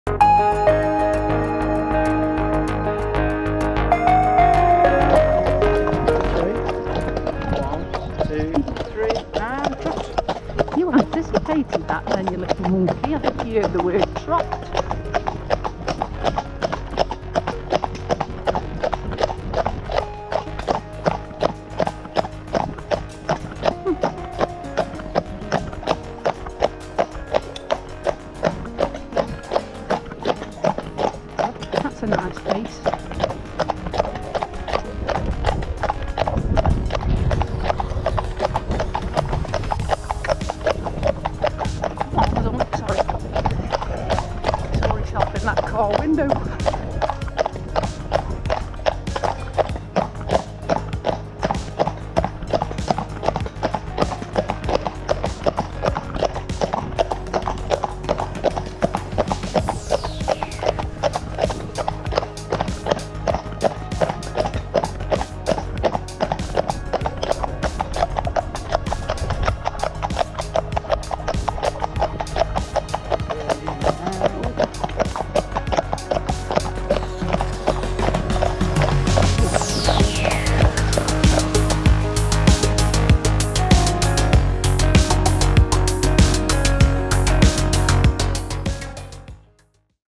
Jodie and Louis synchronized trotting |Ridden Shire Horse
Jodie and Louis practice their synchronized trotting whilst out on their daily hack. Jodie is a ridden Shire Horse.